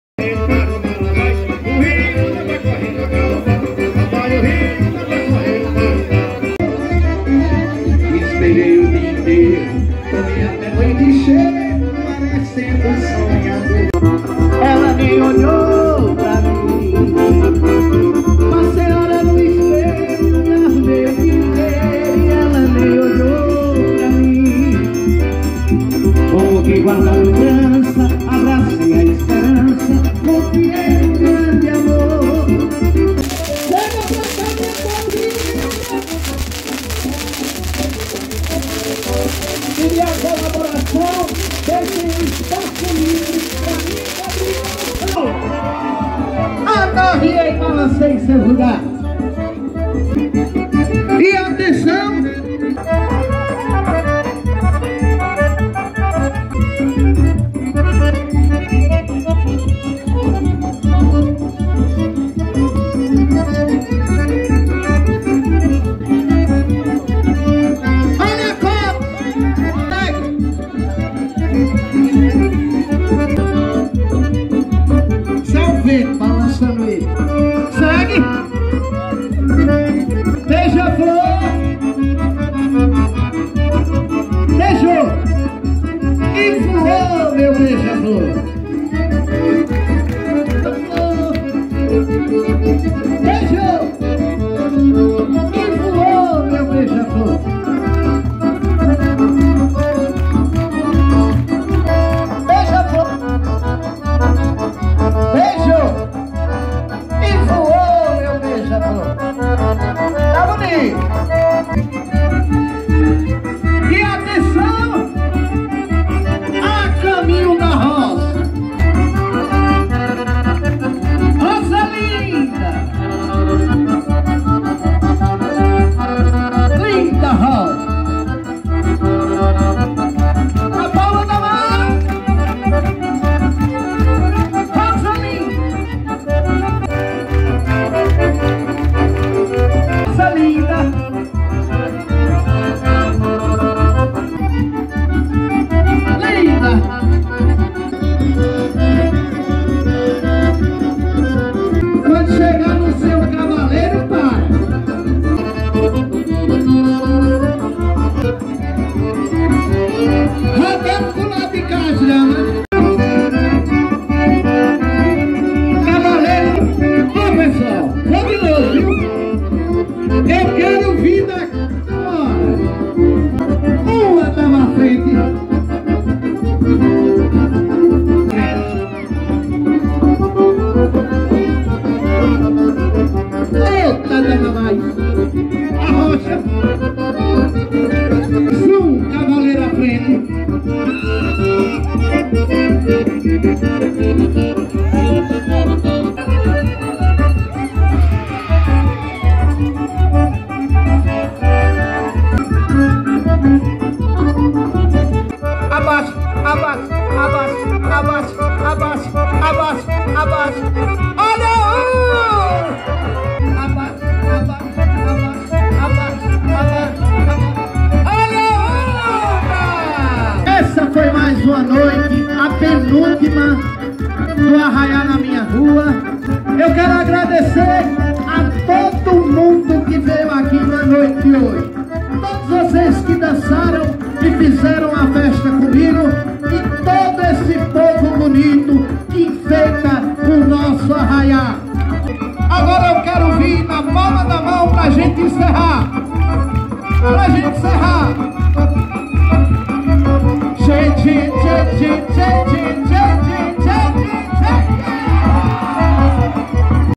Arraiá na Minha Rua 2023 - Loteamento Arco-íris
O sentimento de saudade já começa a aflorar. O antepenúltimo arraiá de 2023 aconteceu no Loteamento Arco-íris, e como sempre a animação estava nas alturas! Viva a nossa cultura!